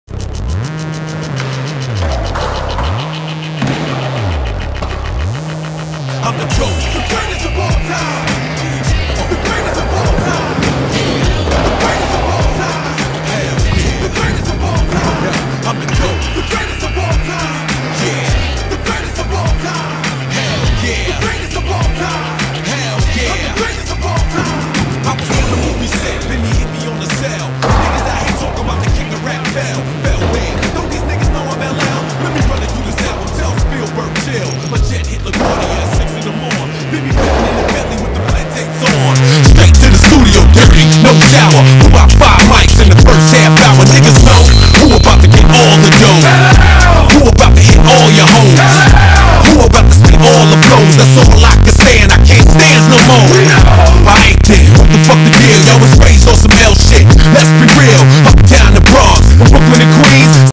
scooters - One Minute At Pioneer
Few banger lines at pioneer skatepark